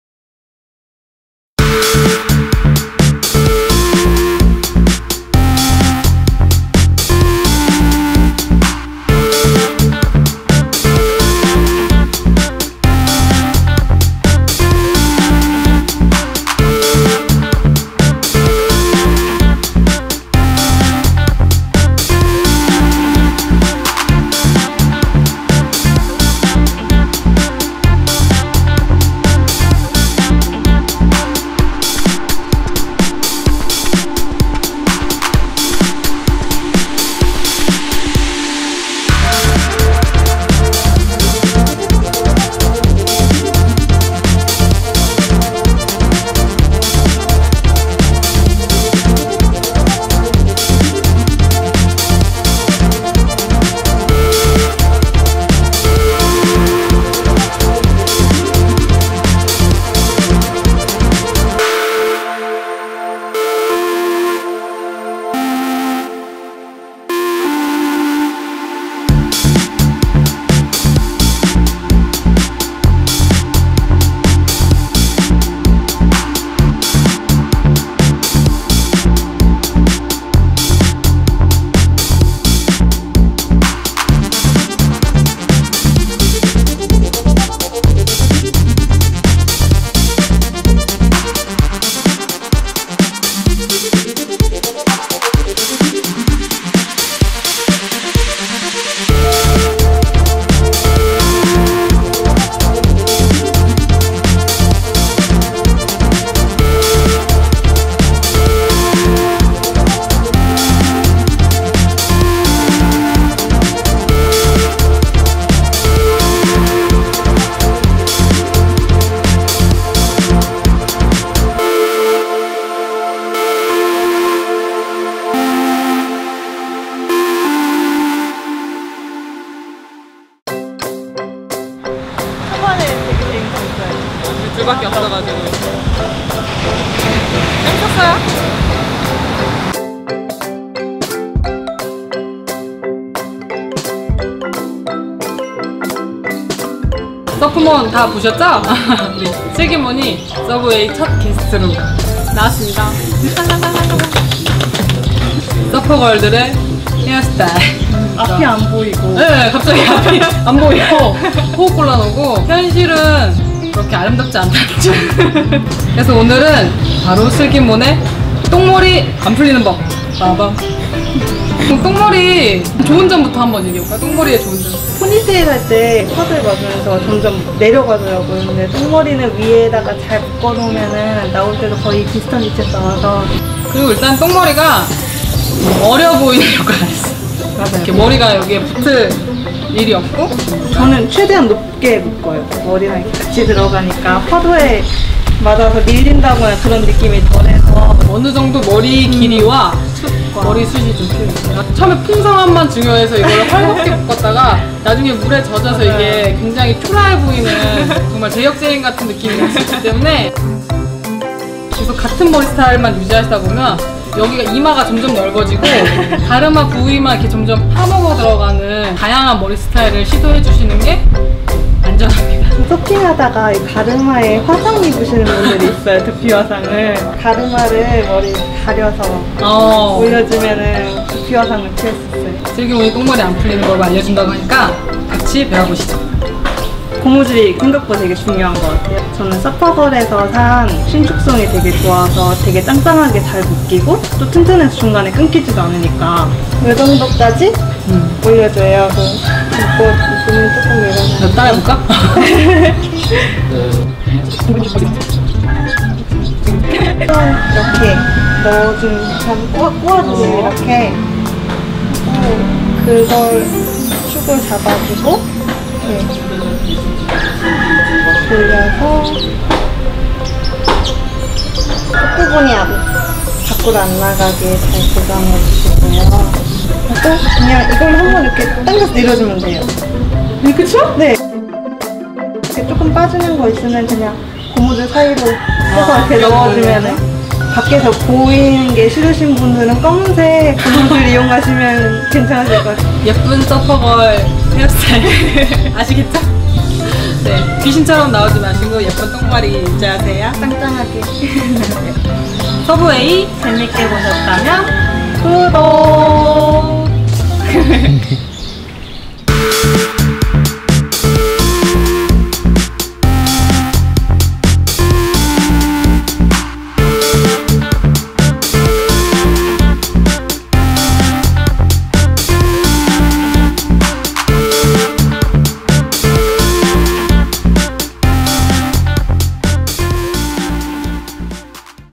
서퍼걸 헤어스타일 | 서핑꿀팁 서부웨이 06
서퍼부부가 알려드리는
서핑꿀팁 서부웨이😎🌊🤙
빈땅빈땅서프몬2 의 슬기몬과 함께한 주제는?
서퍼걸 헤어스타일 빠밤!
거친 발리 파도에도 풀리지 않는 똥머리의 비법!
서퍼걸의 품위를 유지하고 싶다면?!
지금 바로 영상 확인!
서핑에 100% 정답은 없기에
다소 부족한 점이 있을 수 있습니다.
부족한 점, 보충할 점, 공유하고 싶은 이야기들은
답글로 남겨주세요🙏🙏🙏